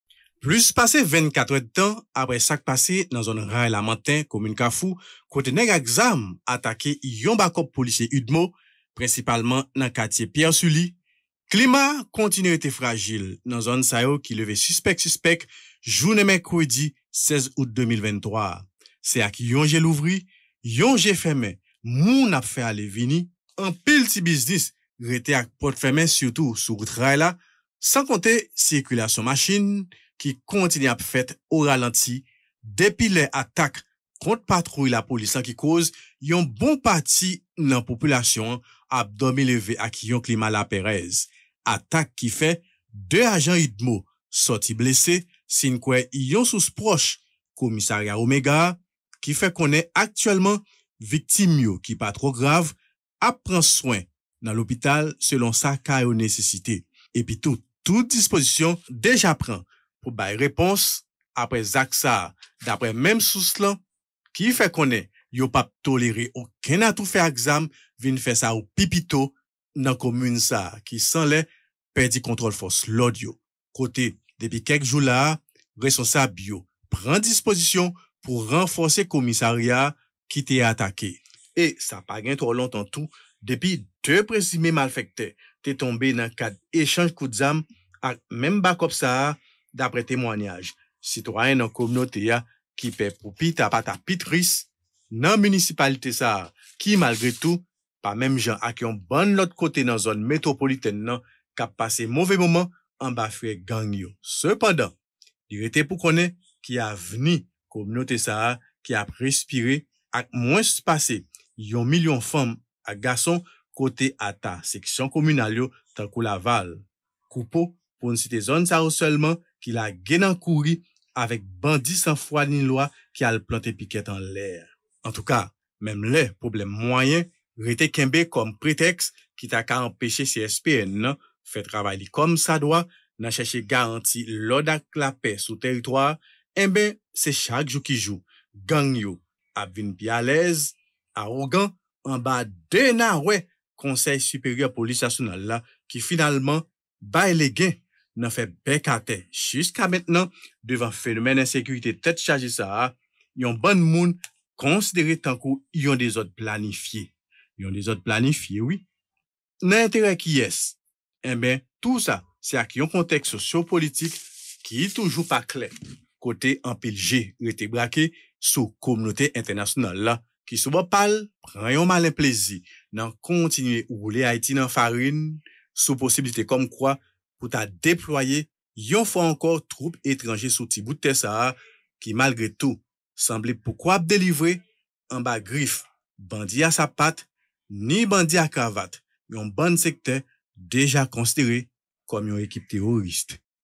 atak bandi ak zam kont yon bakòp lapolis nan zòn ray, lamanten 54 kote 2 ajan UDMO soti blese
Klima toujou rete frajil nan komin kafou yon jou aprè atak bandi ak zam kont yon bakòp lapolis nan zòn ray,lamanten 54 kote 2 ajan UDMO soti blese